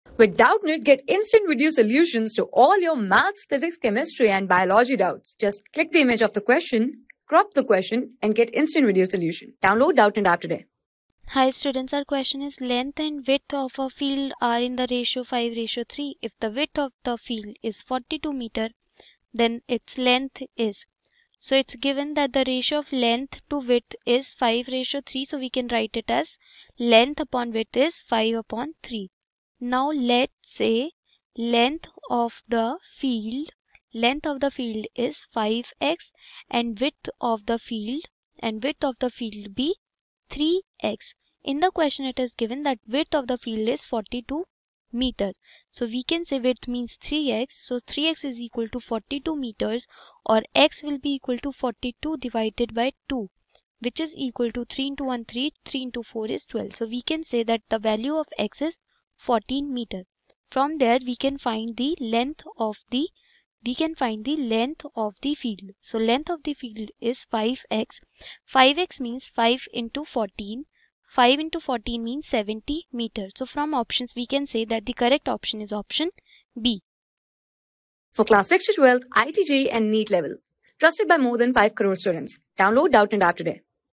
Length and width of a field are in the ratio\n5:3. If the width of a field is 42m, then its leng...
Length and width of a field are in the ratio\n5:3. If the width of a field is 42m, then its length is \n(a) 50m \n(b) 70m (c) \n80m (d) 100m
Class: 6
Subject: MATHS
Chapter: RATIO, PROPORTION AND UNITARY METHOD
Board:CBSE

👉 Have Any Query? Ask Us.

Our Telegram Pages:
class 6
6 class ka math syllabus